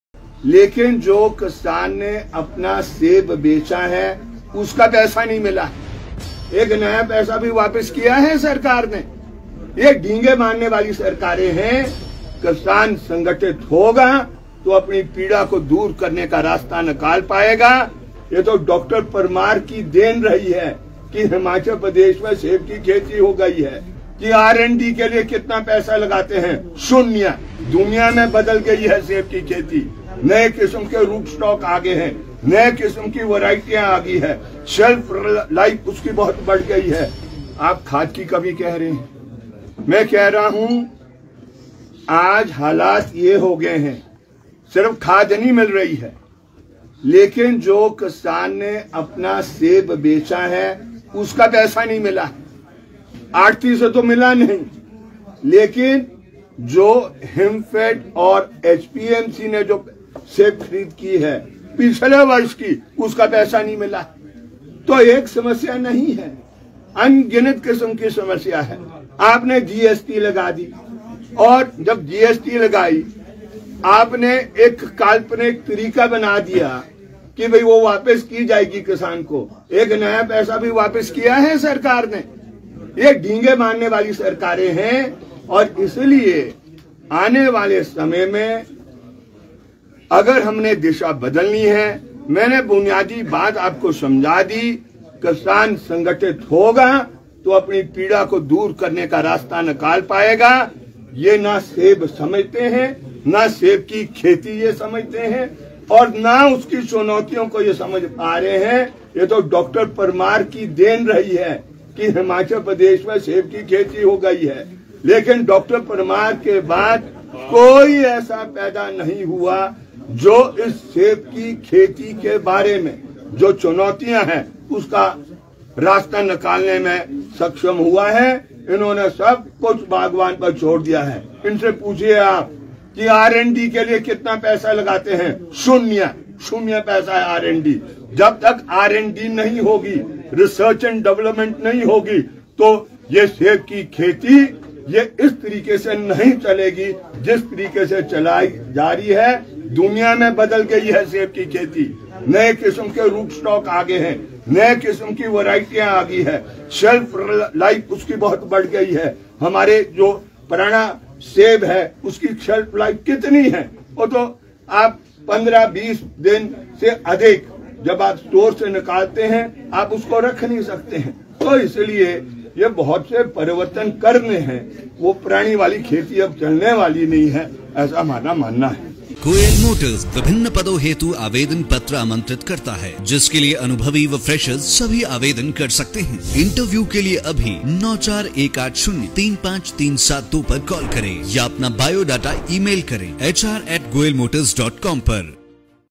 HIMFED और HPMC ने अब तक नहीं दिया बागवानों का पैसा- सिंघा
HIMFED and HPMC have not yet given the money to the gardeners - Singha